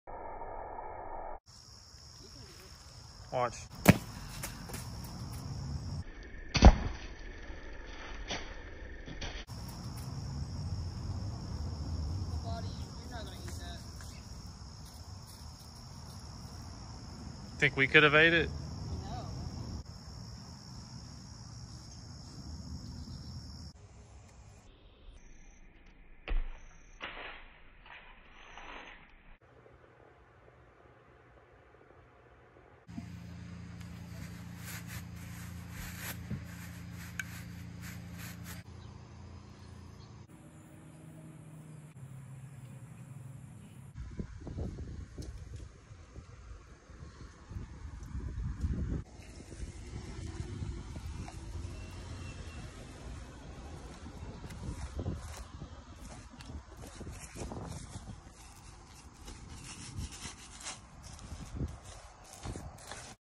Throwing a Rotten Cucumber and other absurdly “fortuitous” shenanigans! (with Oyond Art) 🥒
Ever wonder what an exploding half rotted cucumber looks like? Well here you go. Some science experiments and aesthetic moments caught by yours truly.